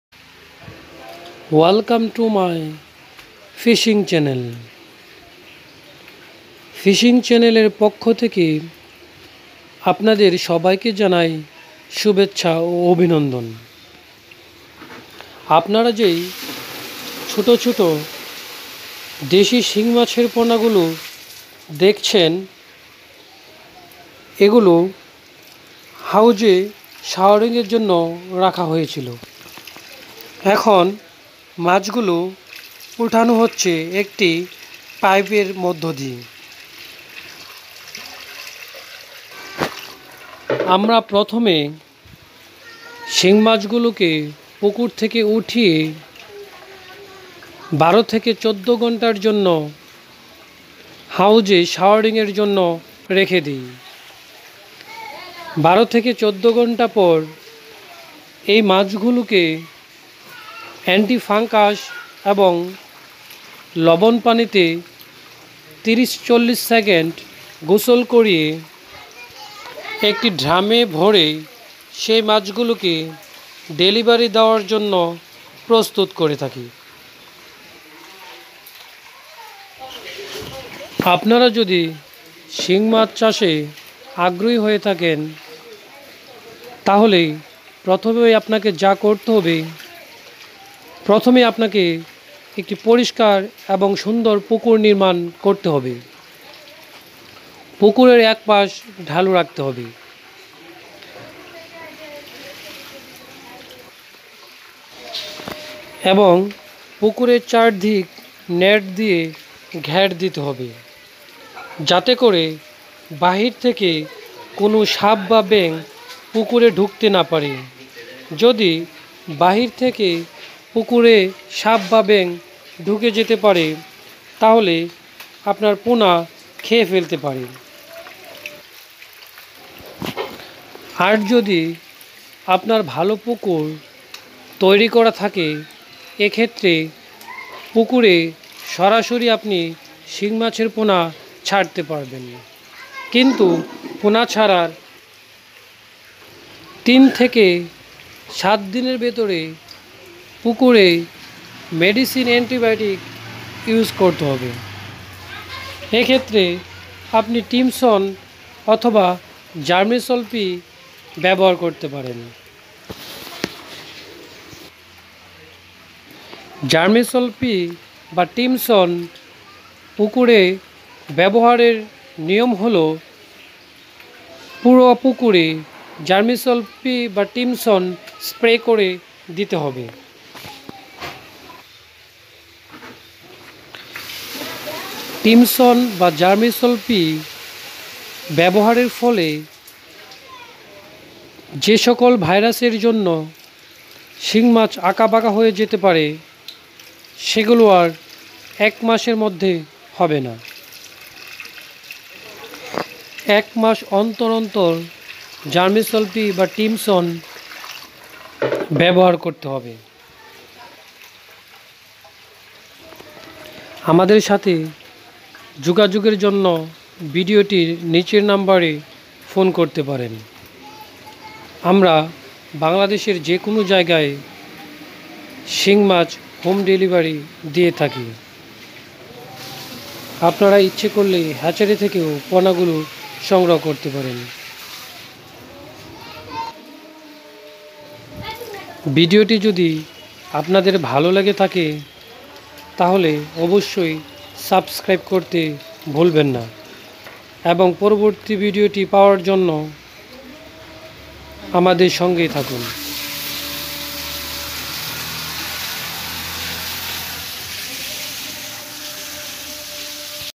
শিং মাছ চাষের জন্য কিছু উপদেশ মূলক কথা | Some tips for cultivating horn fish |যোগাযোগে=01911313020
শিং মাছ চাষ করতে যে সকল টিপস জানতে হয় সেগুলো হল আজকের ভিডিওর মূল বিষয়।
আমরা বাংলাদেশের যেকোন স্থানে শিং মাছের পোনা বিক্রি করে থাকি এবং হোম ডেলিভারি দিয়ে থাকি। আমাদের সাথে যোগাযোগের জন্য ভিডিওটি নিচের নাম্বারে ফোন করতে পারেন।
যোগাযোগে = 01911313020